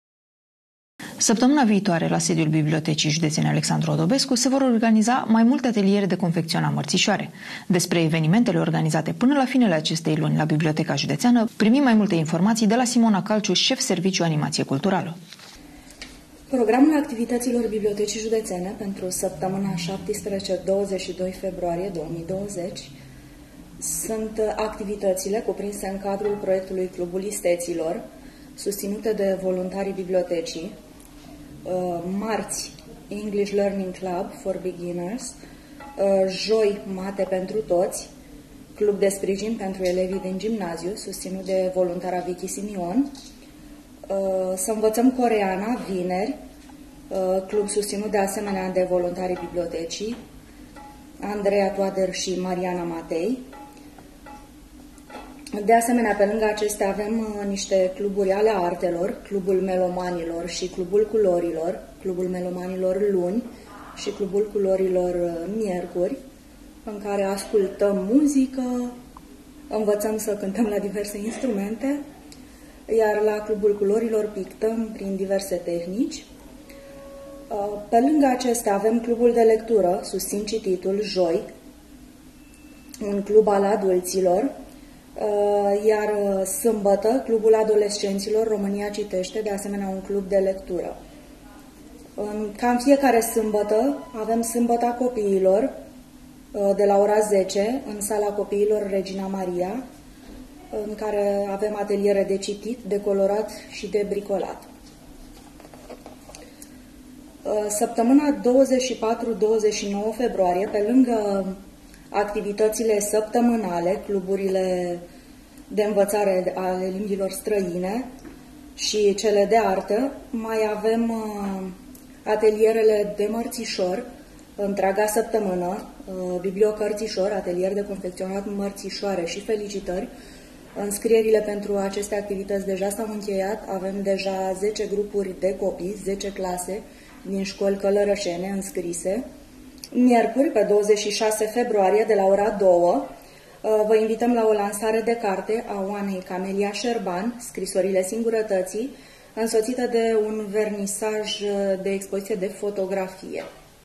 Știri Locale - Ateliere de confecționat mărțișoare la Biblioteca Județeană
La sediul BJ „Alexandru Odobescu” se vor organiza ateliere de confecționat mărțișoare.